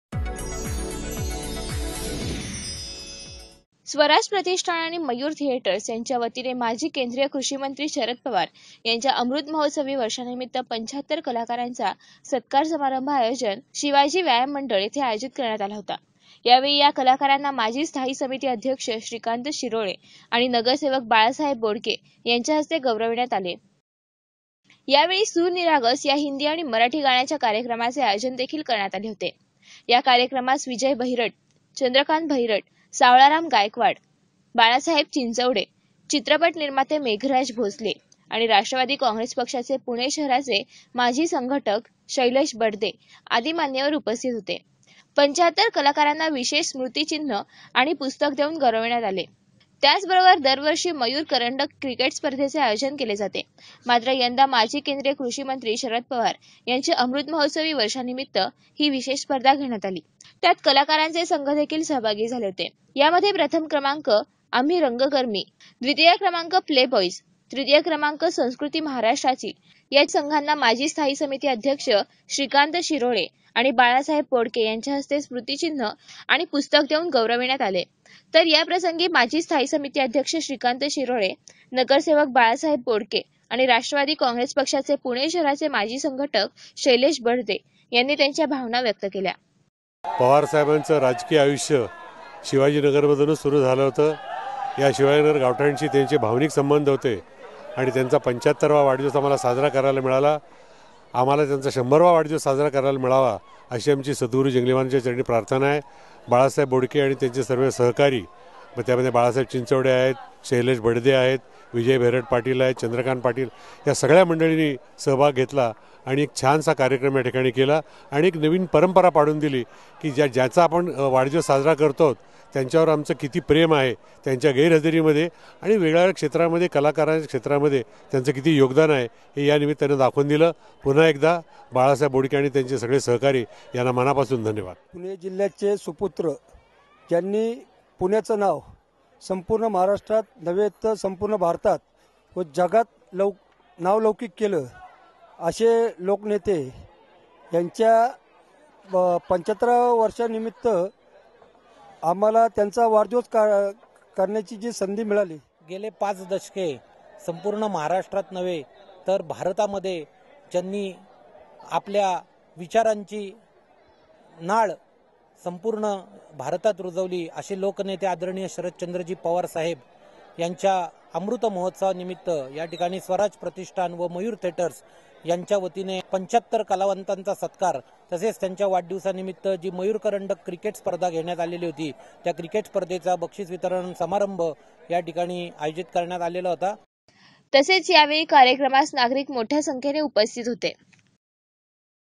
75 Artist's given awards
On the occassion of Hon Sharad Pawar's b'day, 75 artists across Maharashtra were awarded